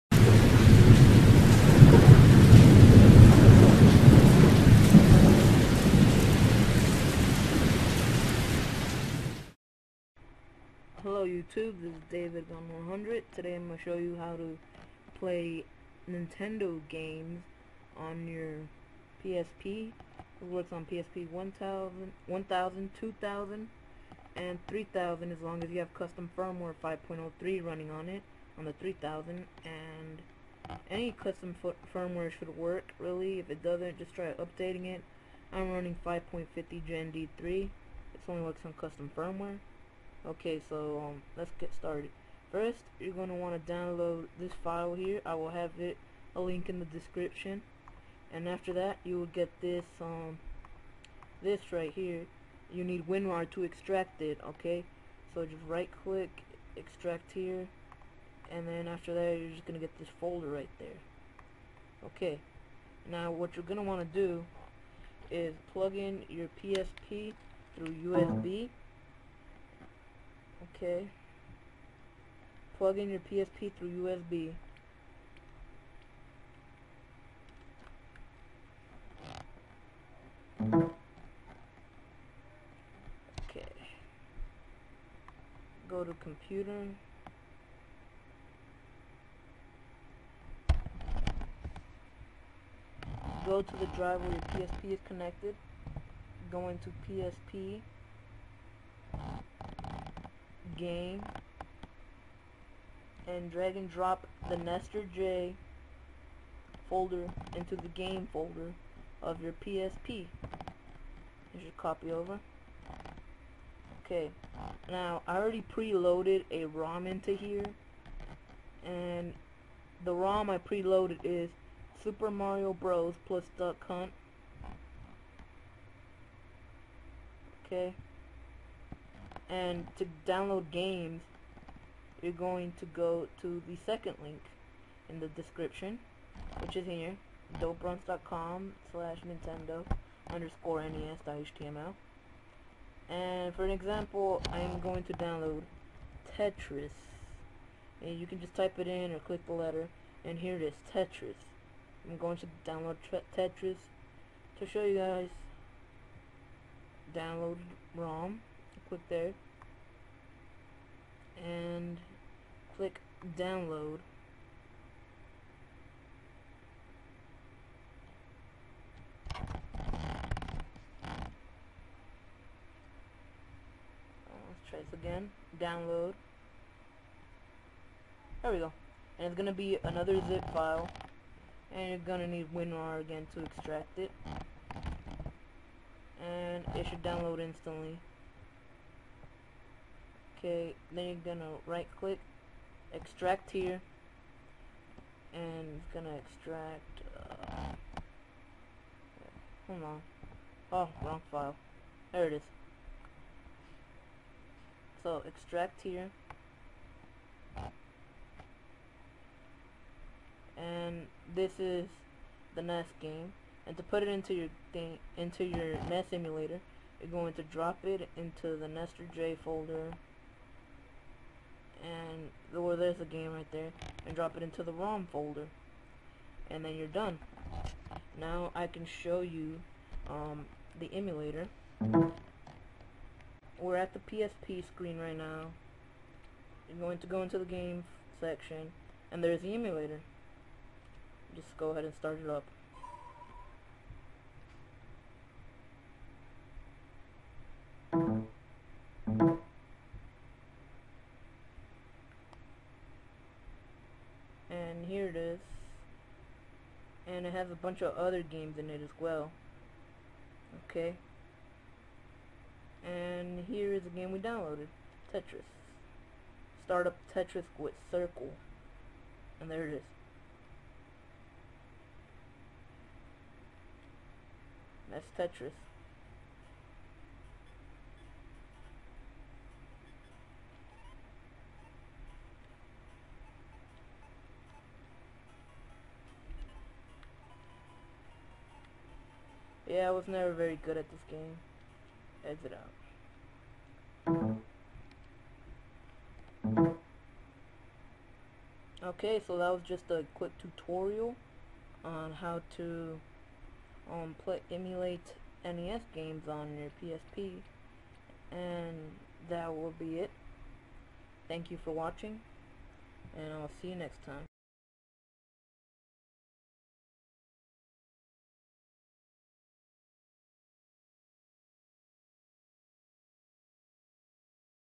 Tutorial:PSP How to play Nes on your psp NesterJ [HD]
Update 10/22/10***fixed problem with not being able to find game in roms!
You need custom firmware for this 3.71 m33 or higher I have 5.50 gen D3.